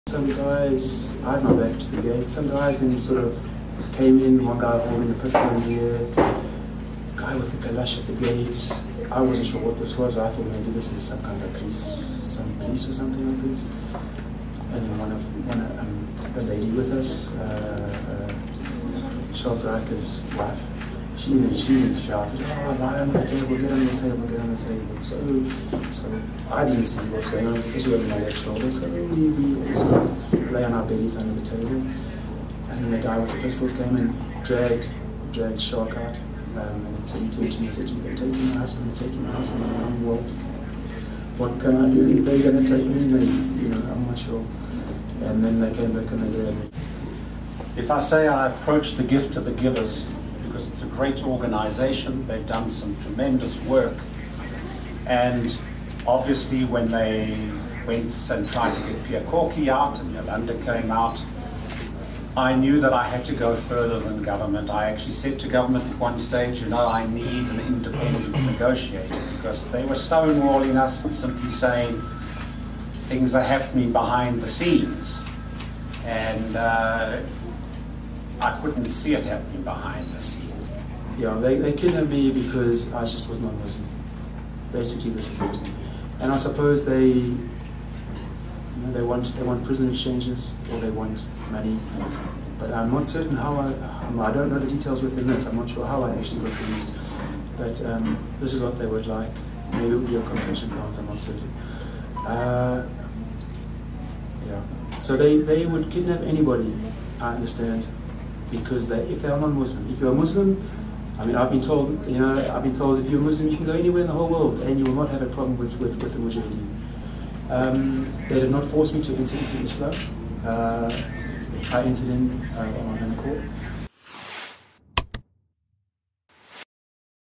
'They kidnapped me because I was not Muslim' - Stephen McGown
Stephen McGown addresses the media recounting the events of his kidnapping at the Gift of the Givers foundation in Bramley View on Thursday afternoon.